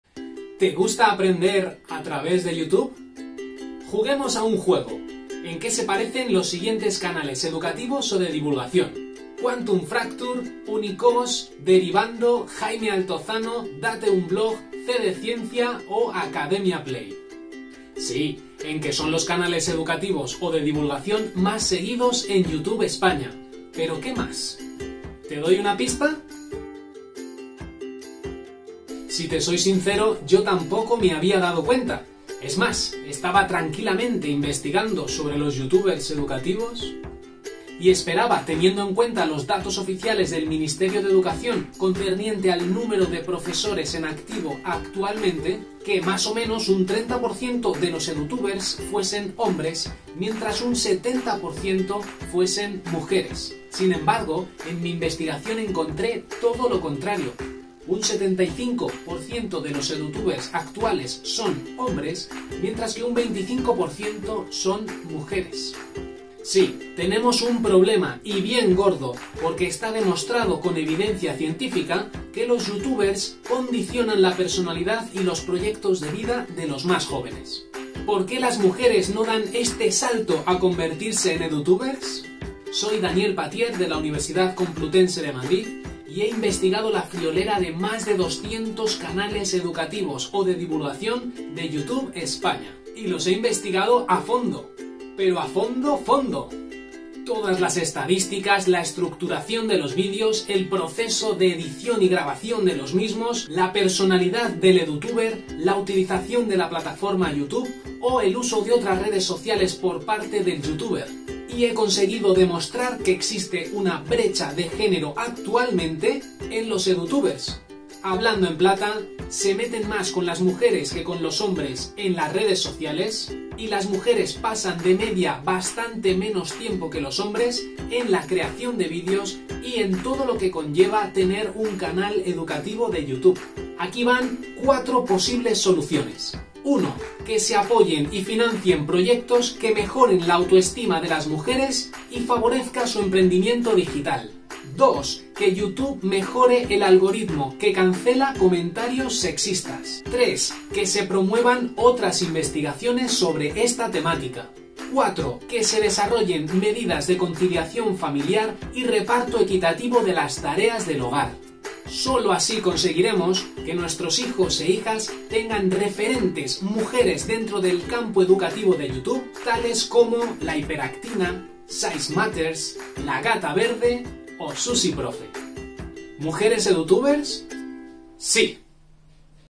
¿Mujeres edutubers?
Este vídeo ha sido seleccionado en el XI Concurso de Divulgación Científica UCM otorgándole el segundo premio en la categoría "Youtuber".

Derechos de autor: Daniel Pattier

Artículo científico "The Gender Gap Among EduTubers and the Factors Significantly Influencing It"